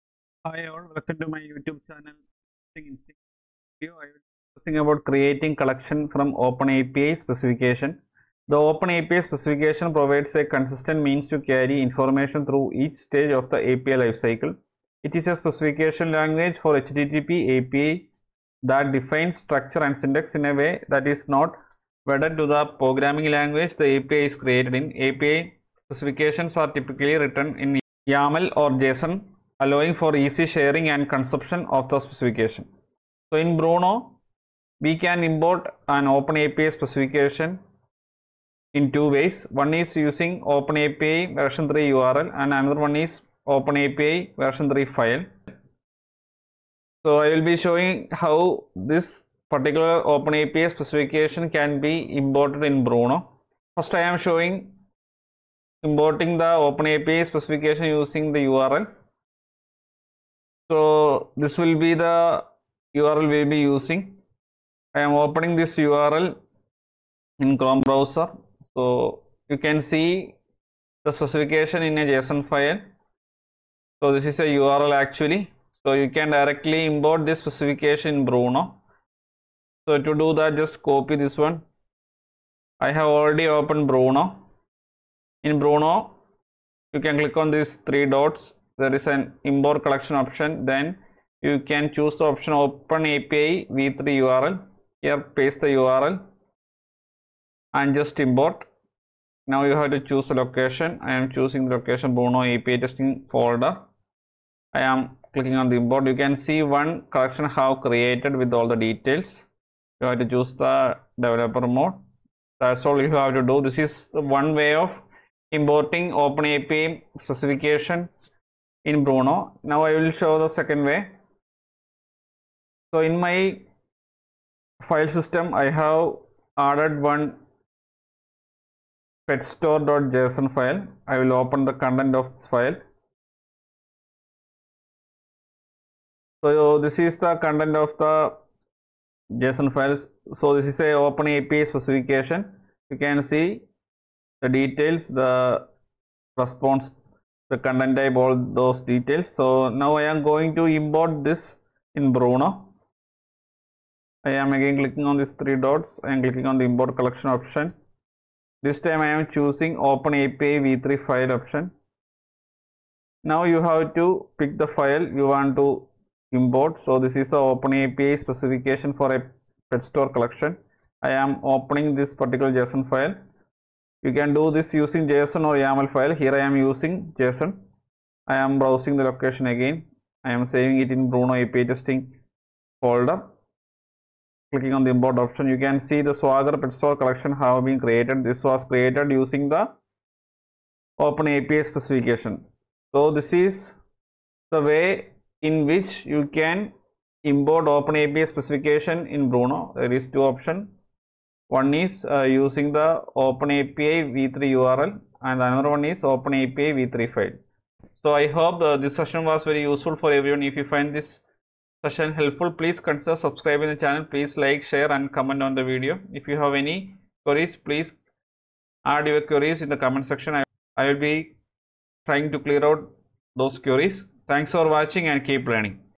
#60 | Create Collection OpenAPI | Bruno API Client | Rest API Testing tutorials for Beginners
The video describes about create collection from OpenAPI Specification. The OpenAPI Specification (OAS) provides a consistent means to carry information through each stage
of the API lifecycle. It is a specification language for HTTP APIs that defines structure
and syntax in a way that is not wedded to the programming language the API is created in.
API specifications are typically written in YAML or JSON, allowing for easy sharing
and consumption of the specification.

This can be done in 2 ways in Bruno

OpenAPI V3 URL
OpenAPI V3 File

Video chapters:
0:00 Introduction
1:23 Demo - Create Collection from OpenAPI Specification